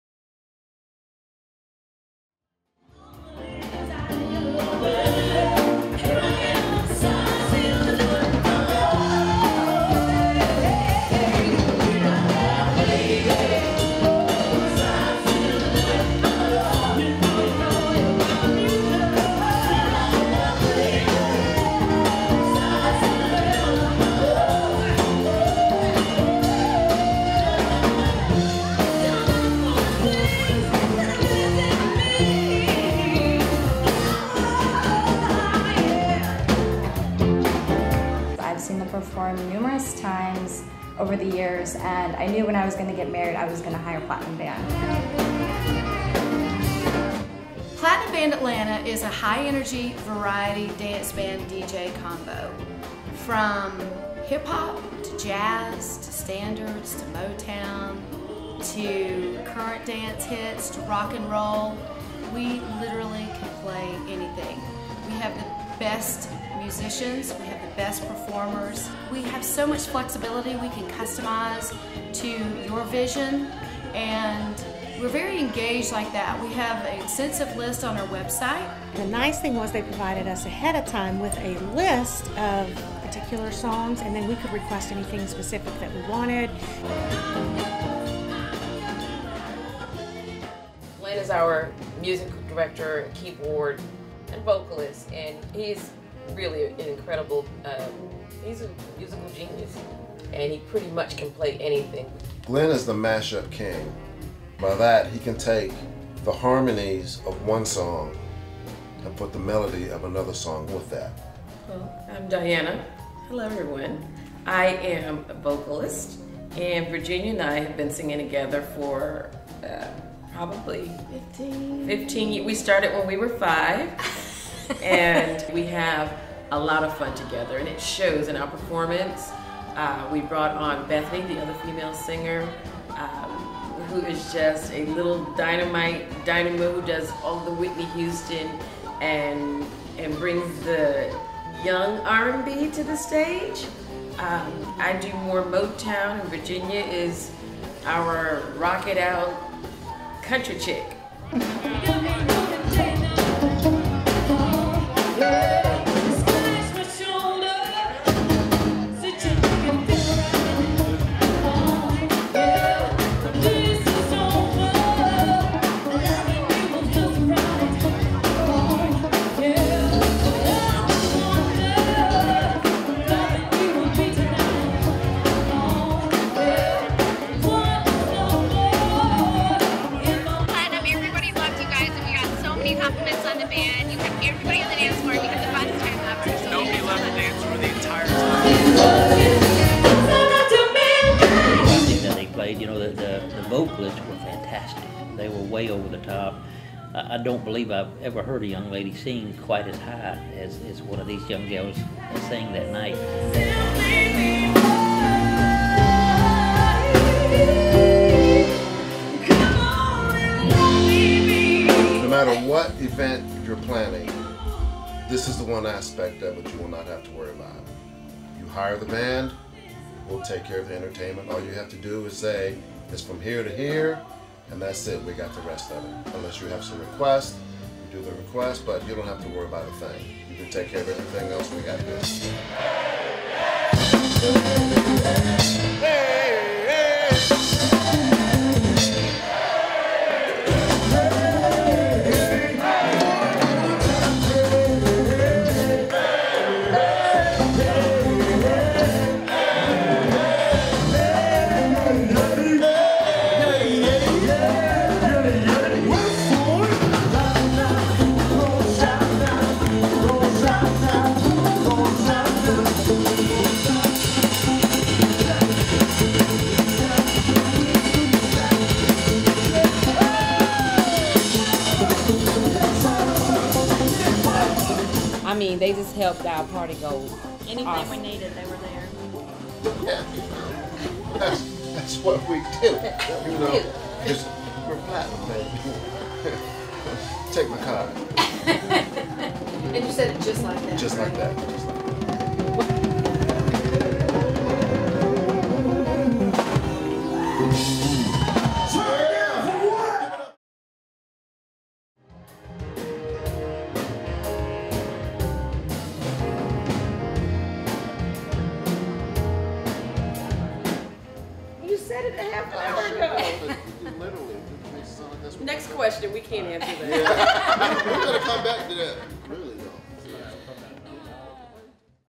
Authentic! All Live! Platinum Band Atlanta Promo Video
Check out Platinum Promo Video. Platinum plays all music live with highly skilled musicians and singers.